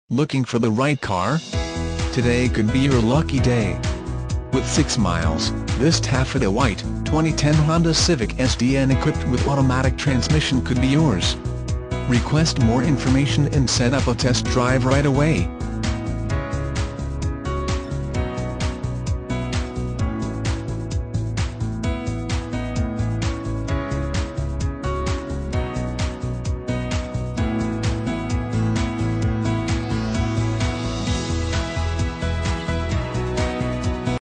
2010 HONDA CIVIC SDN
2010 HONDA CIVIC SDN Littleton CO

Looking for the right car? Today could be your lucky day. With 6 miles, this Taffeta White, 2010 Honda Civic Sdn equipped with automatic transmission could be yours. Request more information and set up a test drive right away.